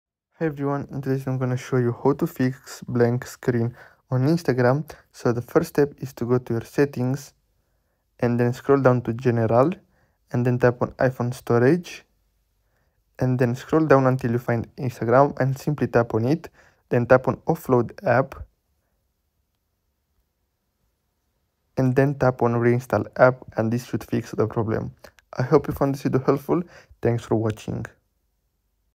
How To Fix Blank Screen On Instagram (Full Guide)
This quick video explains how to fix blank screen on instagram.